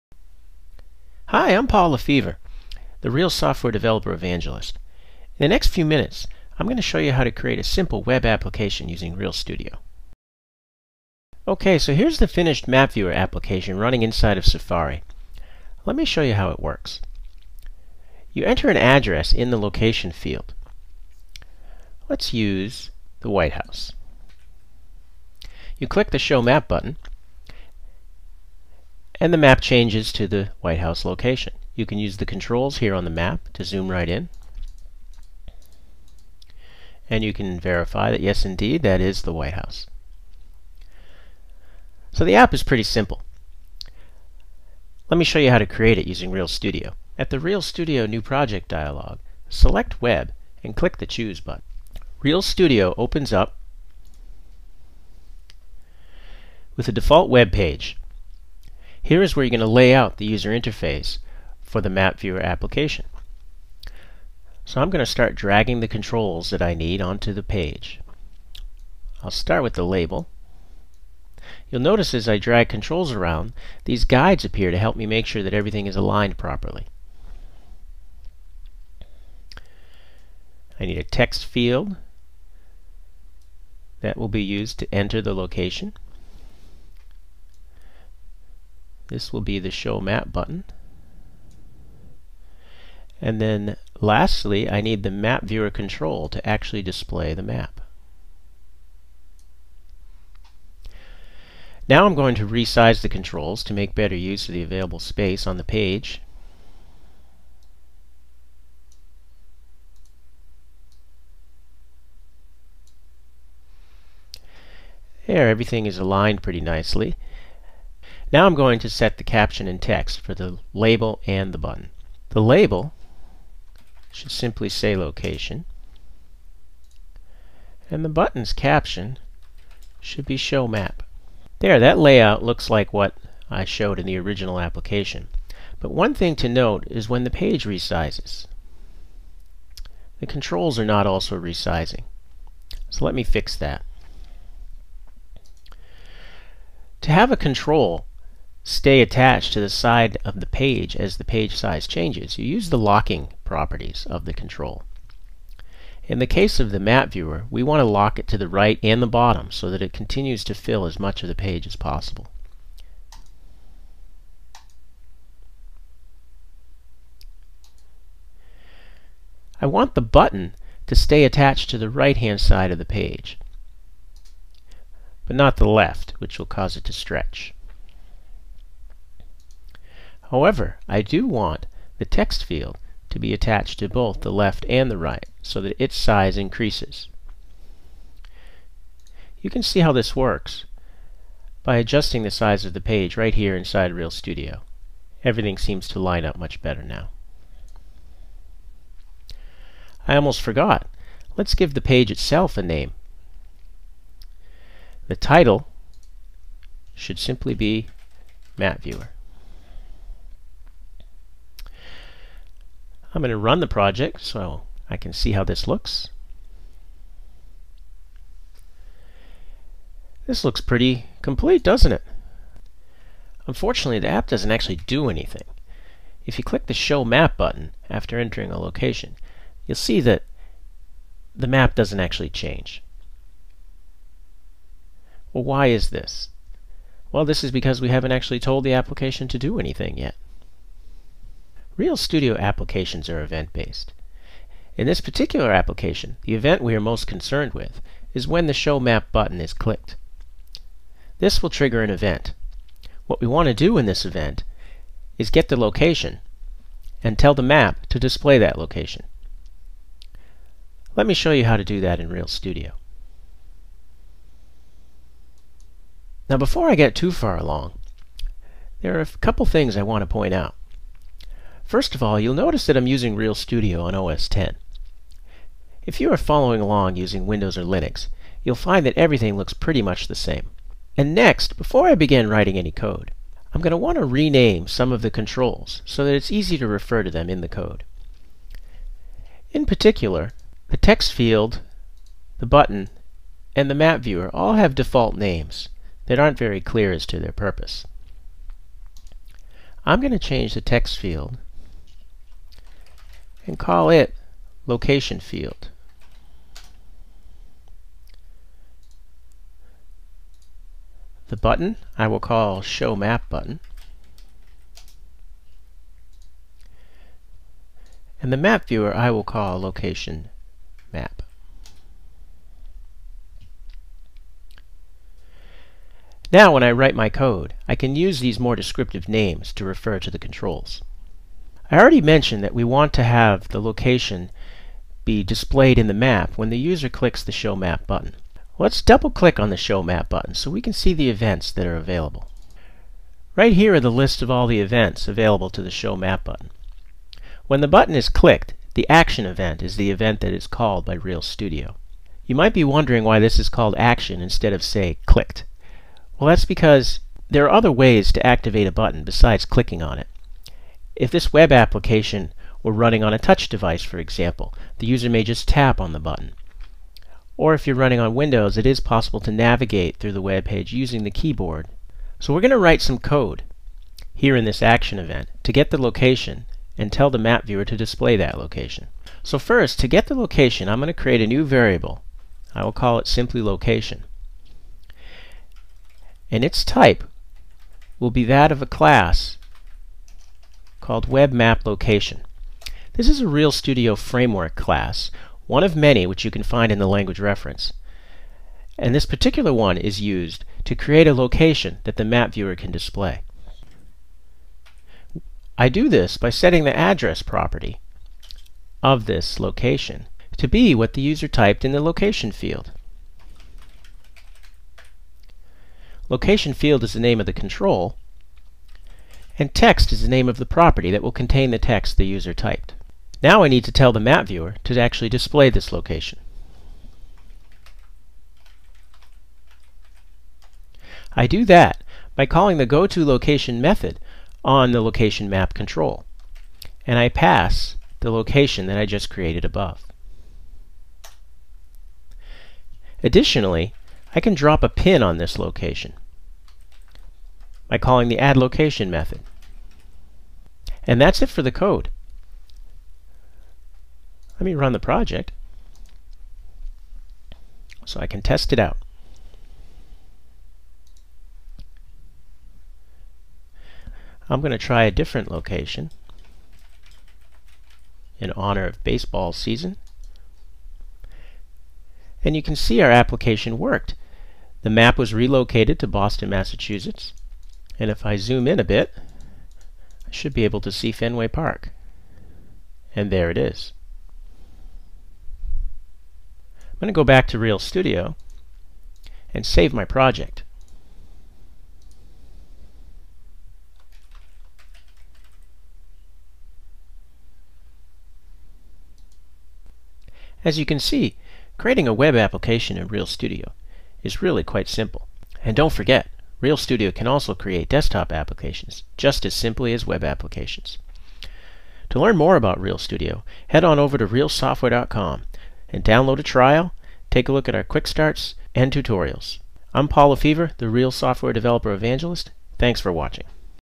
Real Studio QuickStart: Creating a Web Application
In this video Paul uses Real Studio to shows you how to create MapViewer, a simple web application to display map locations on a web page.